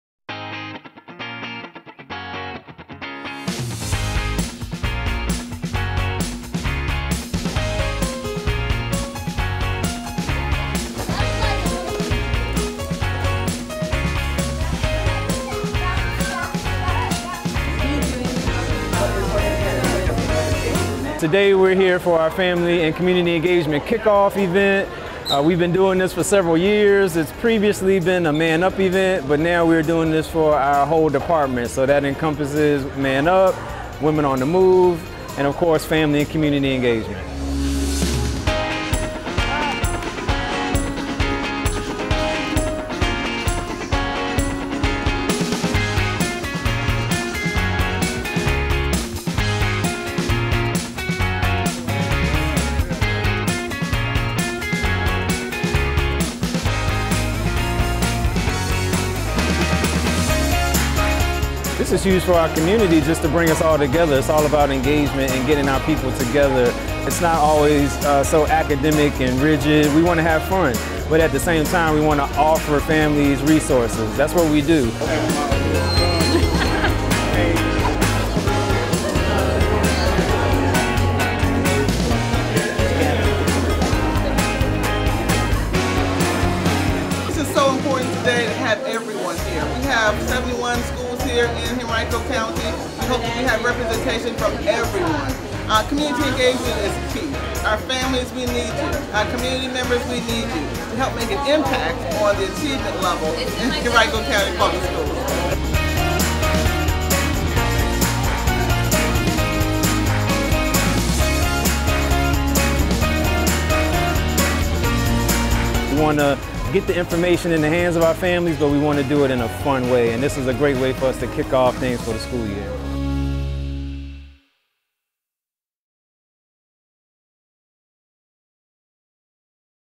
Family Engagement Kickoff Event
HCPS Family Engagement Department recently held it's Kickoff event at Wilder Middle School.